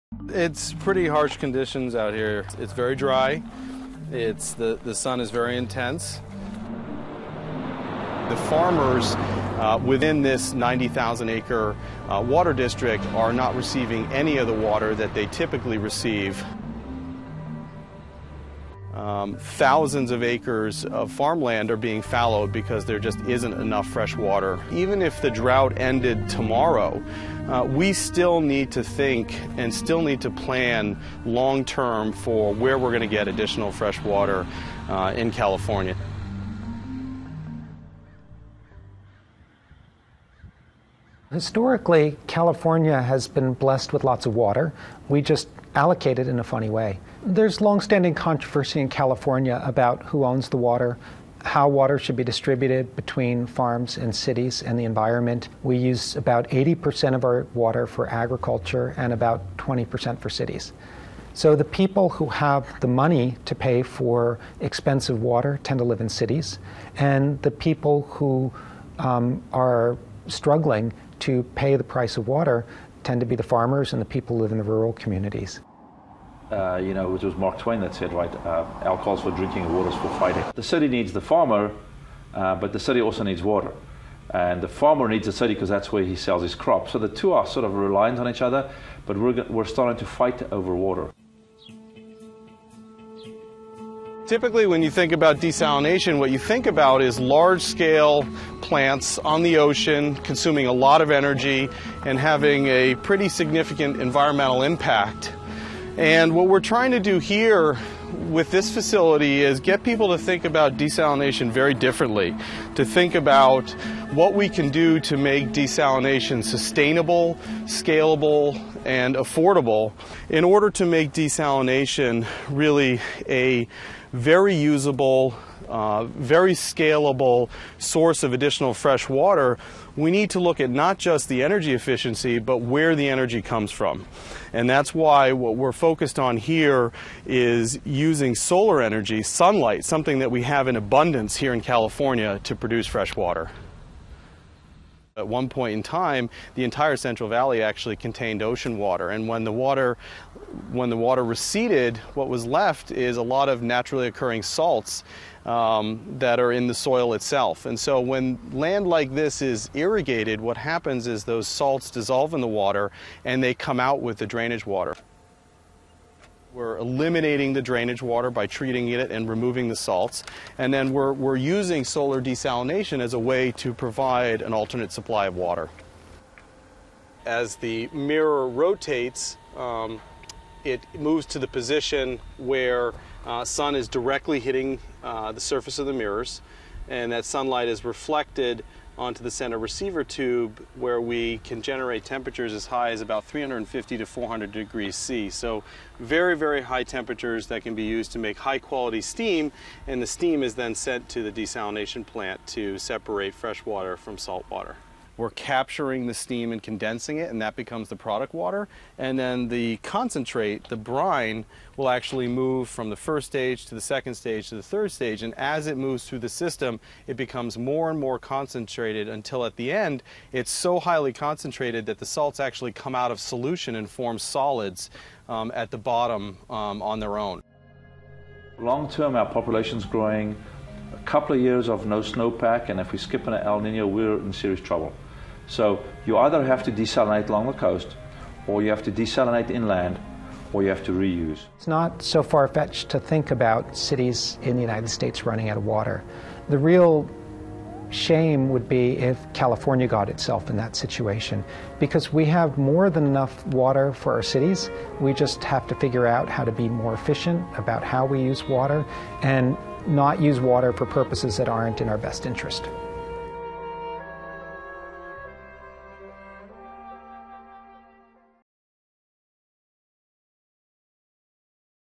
Drought, Solar Energy and Seawater
MSNBC production on the use of solar energy to desalinate sea water in the western US.